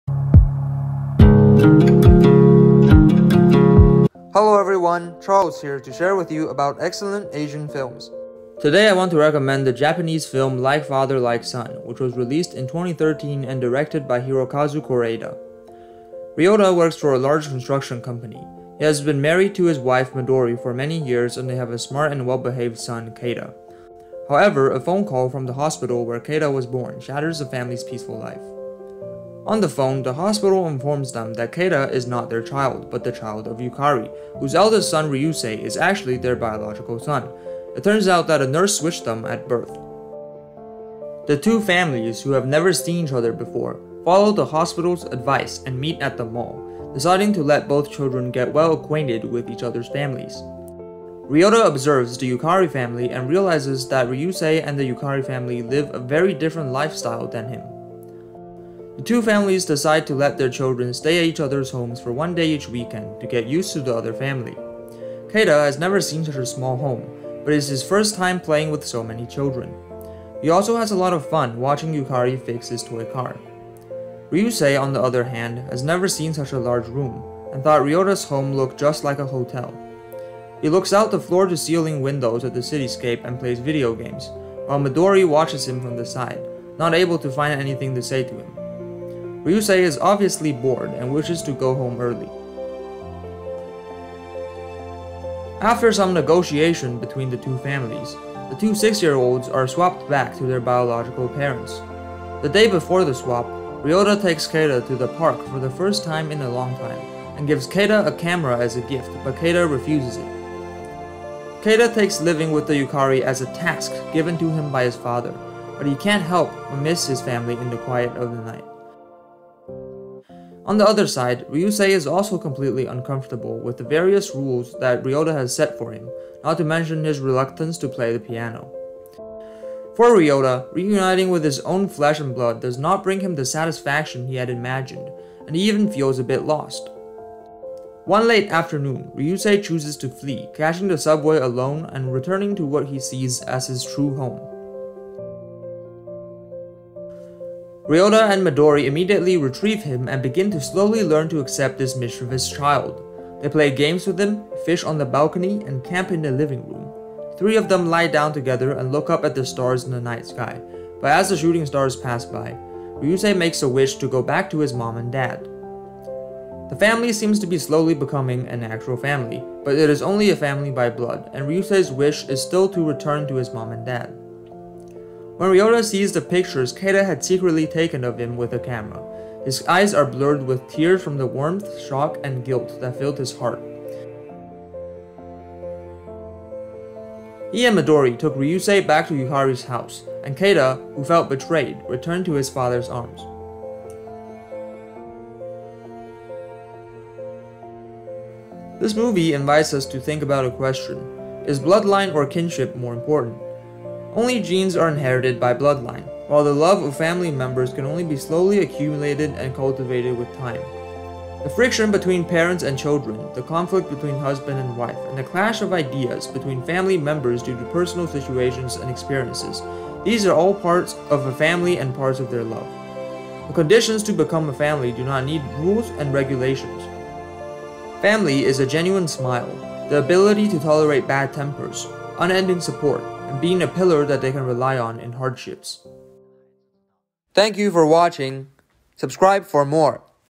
Best Asian Movies | Japan: Like Father, Like Son | High School Student Reviews
Like Father, Like Son is a 2013 Japanese film directed by Hirokazu Kore-eda. Centered around two families whose children were swapped at birth, the movie invites us to think about a question: is bloodline or kinship more important?